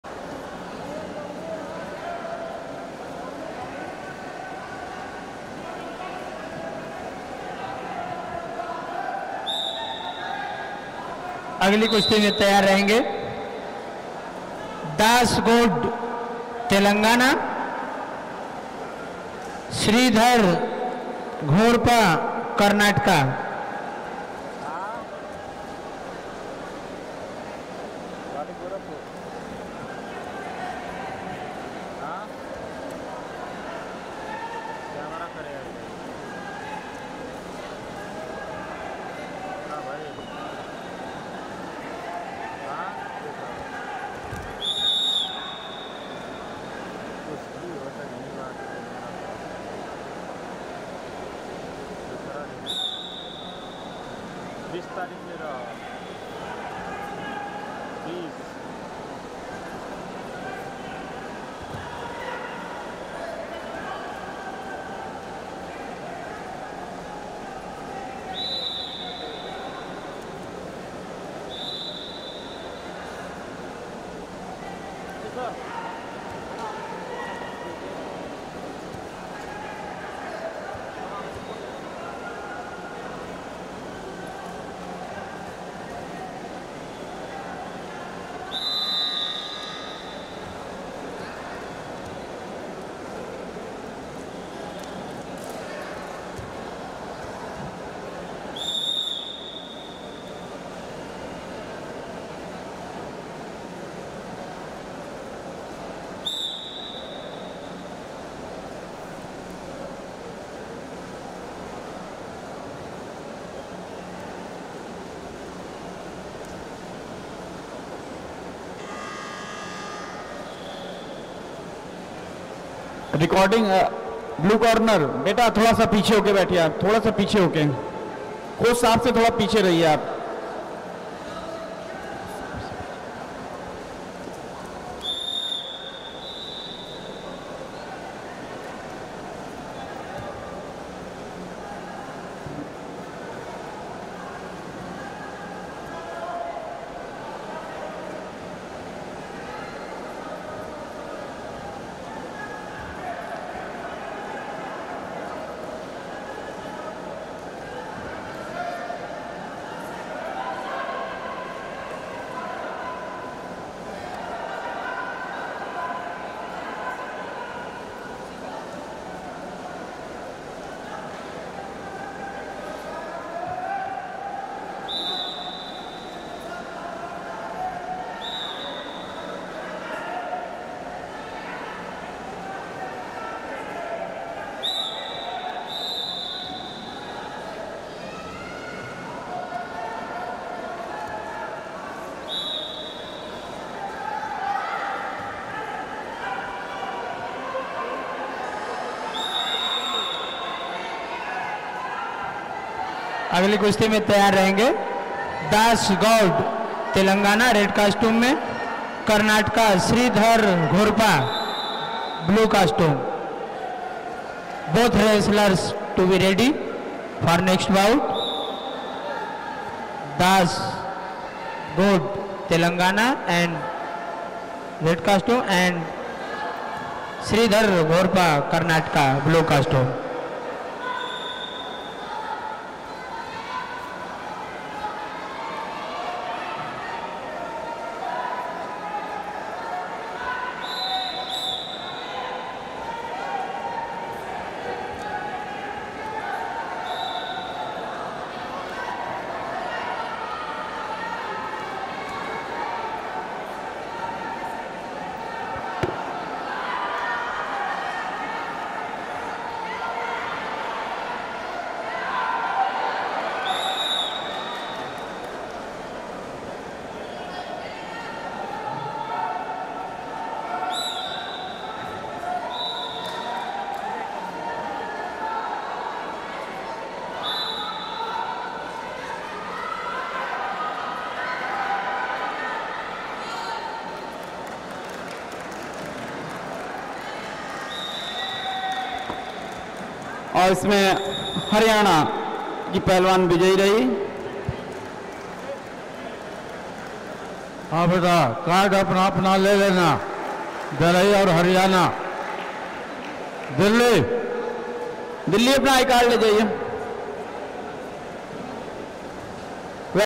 FS 44 | BOUT 129 | DEL KARTIK RANA VS UP HIMANSHU YADAV | 68th NATIONAL SCHOOL WRESTLING CHAMPIONS
68वीं राष्ट्रीय विद्यलयीय खेल (कुश्ती) प्रतियोगिता 2024-25

14 वर्ष आयु वर्ग बालक/बालिका 16 से 20 नवम्बर 2024

गुरु गोरक्षनाथ की तपोभूमि गोरखपुर आगमन पर आपका हार्दिक अभिनन्दन है।

बीर बहादुर सिंह स्पर्पोर्ट्स कालेज, गोरखपुर

ORGANIZED BY - SECONDARY EDUCATION DEPTT, MEERUT DIVISION (UP)